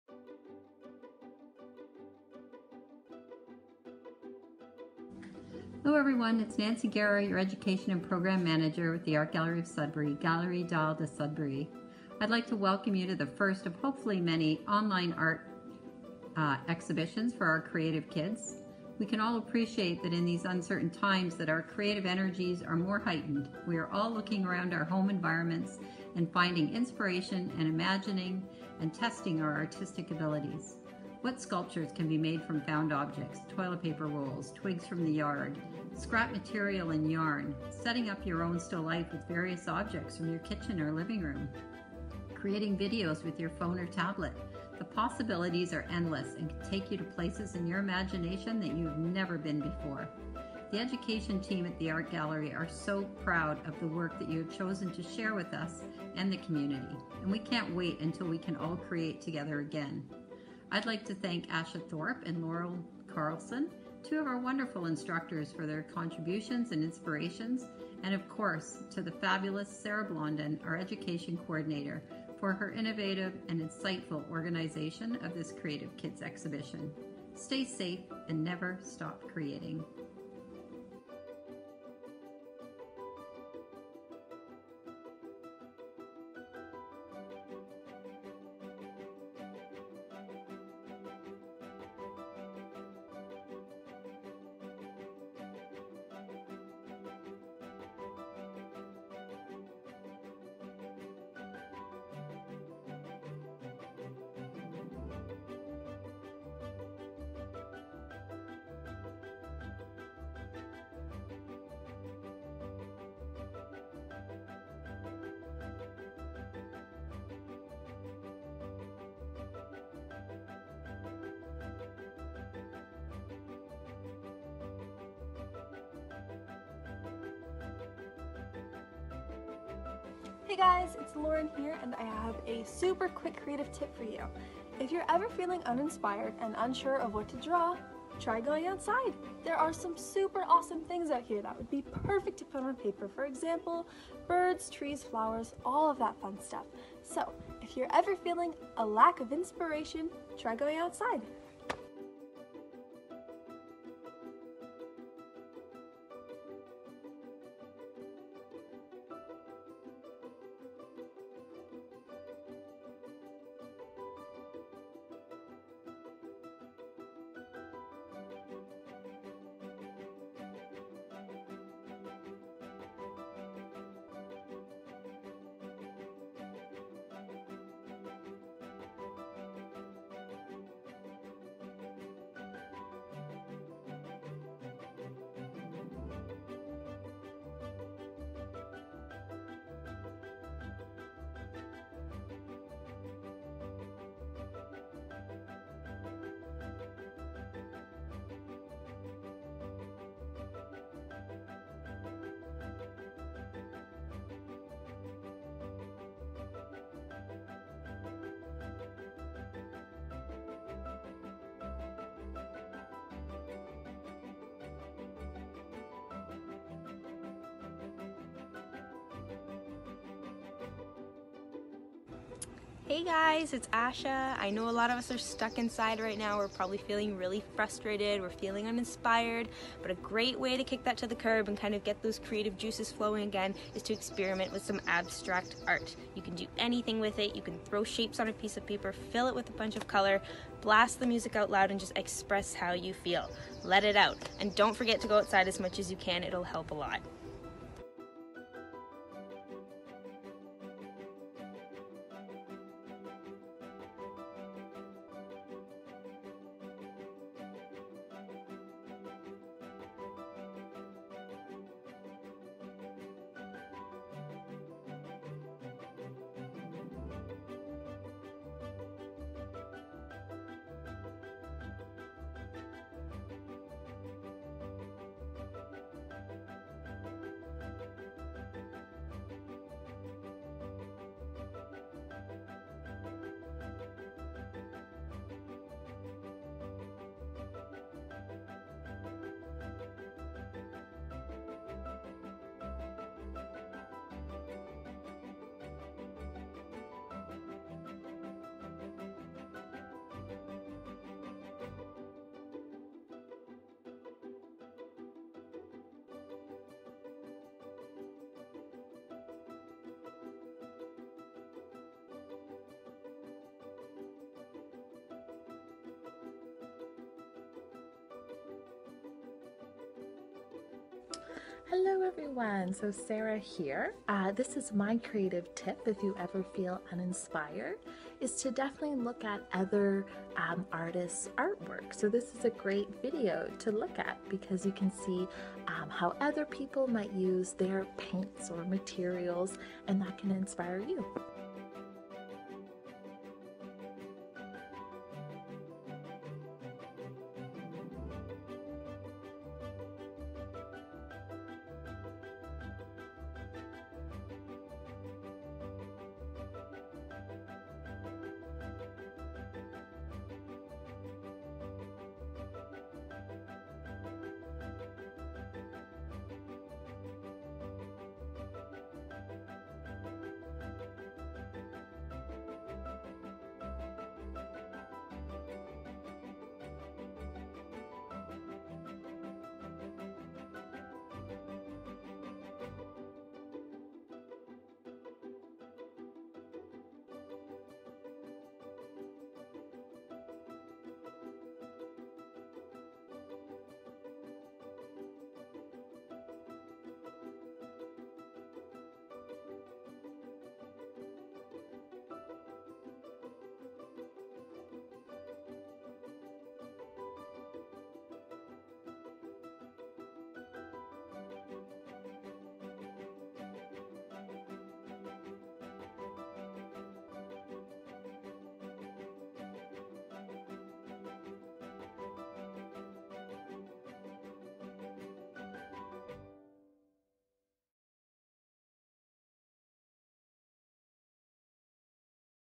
CREATIVE KIDS EXHIBITION / EXPOSITION LES ENFANTS CRÉATIVE
Thank you so much to everyone who participated, we had so much fun looking at all the amazing art work! :)

Merci beaucoup à tous ceux qui ont participé, nous avons eu beaucoup de plaisir à regarder toutes ces œuvres d'art incroyables! :)

Thank you to the Art Gallery of Sudbury, Greater City of Sudbury, Canada Council for the Arts
and the Ontario Arts Council.

Merci à la Galerie d'art de Sudbury, Grand Sudbury, Conseil des art du Canada et le Conseil des arts de l'Ontario.